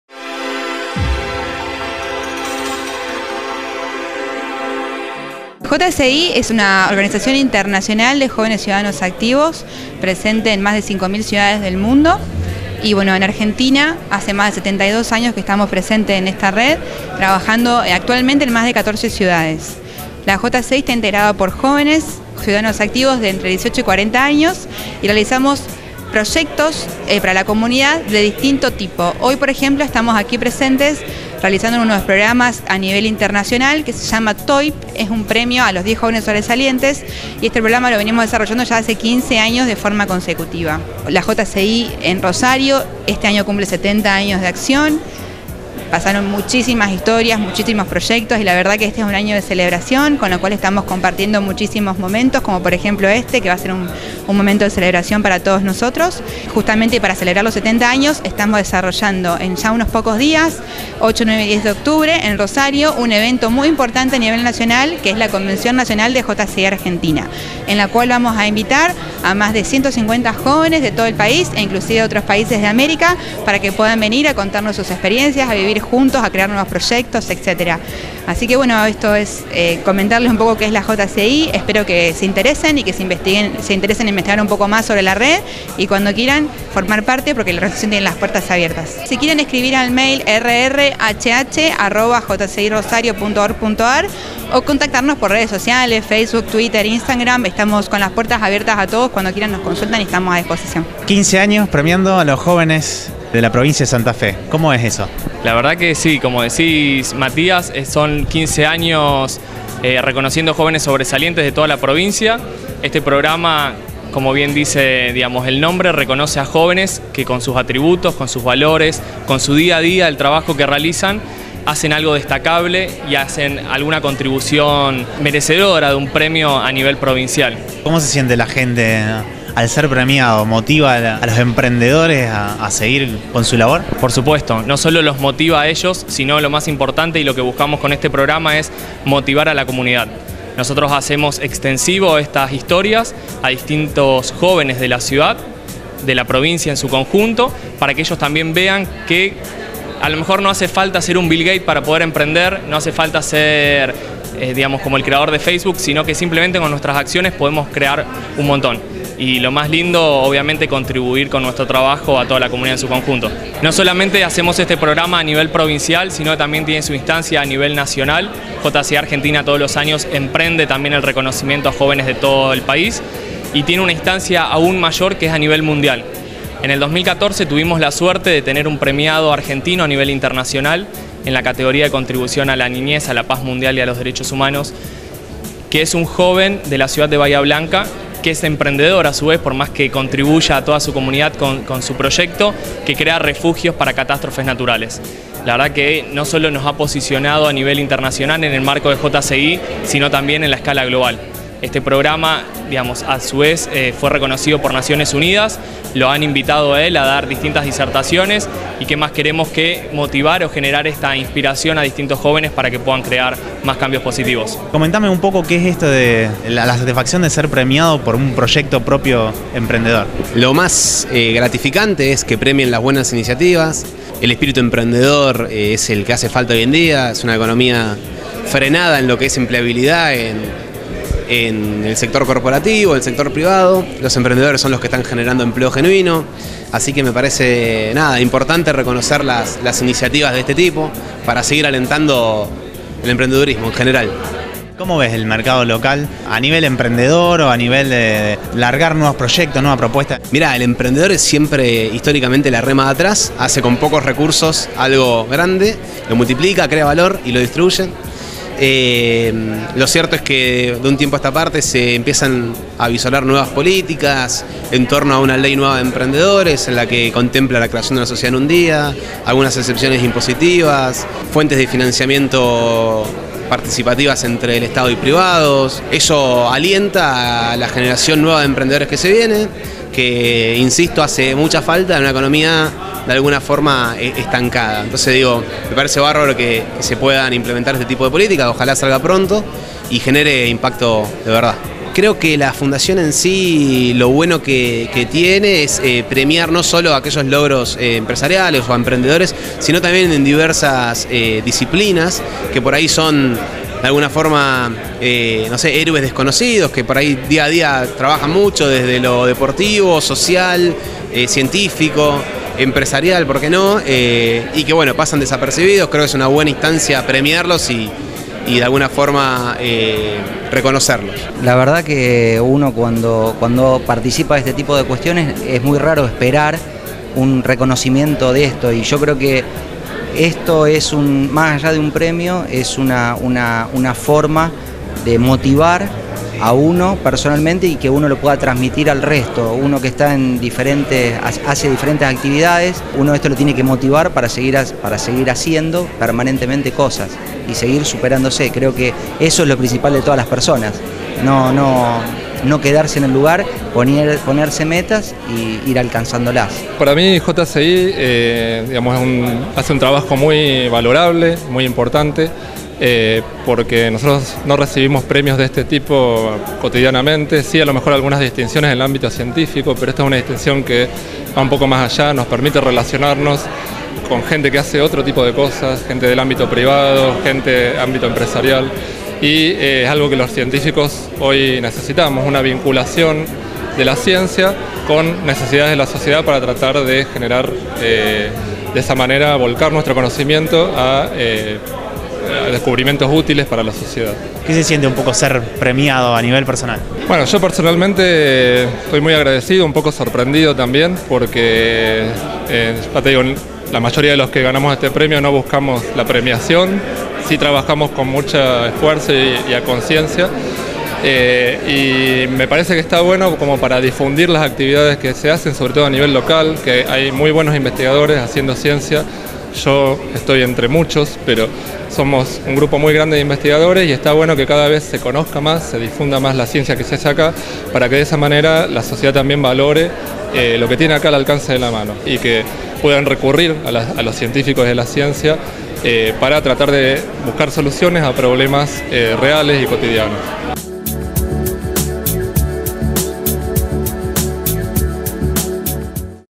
TOYP 2016 JCI Rosario premia a jovenes emprendedores
En el marco del TOYP Santa Fe 2016 entrevistamos a miembros de JCI Rosario así como a los jóvenes premiados en esta edición. Los jóvenes seleccionados representan el progreso cultural, económico y personal de nuestra época, motivando a la sociedad con su entusiasmo emprendedor.